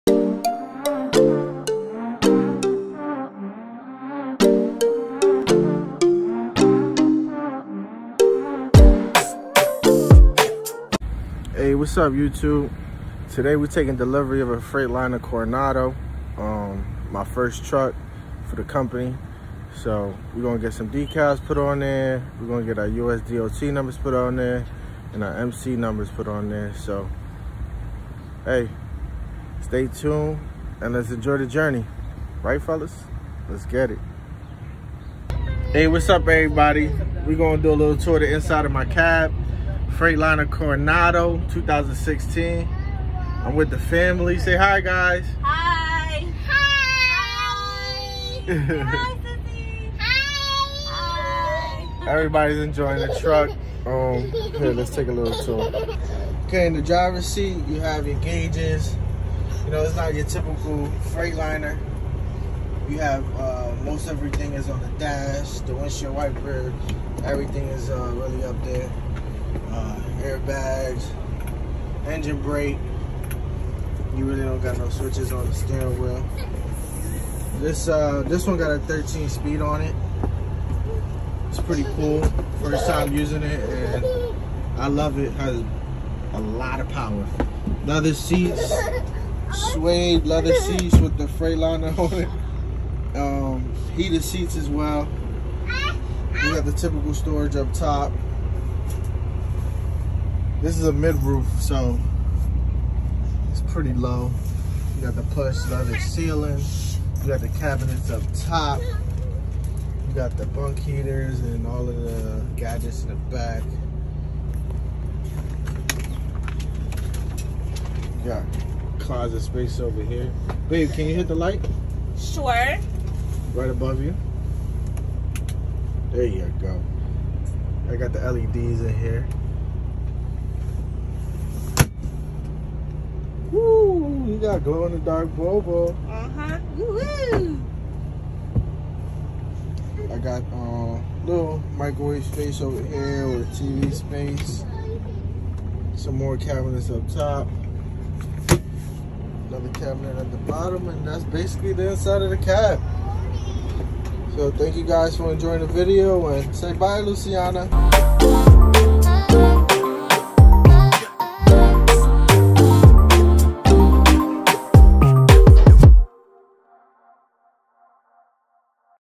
Taking delivery of my first semi for my trucking company!!
Inside tour of my 2016 freightliner coronado. Was a long journey getting to this point!! Take a look and enjoy the video.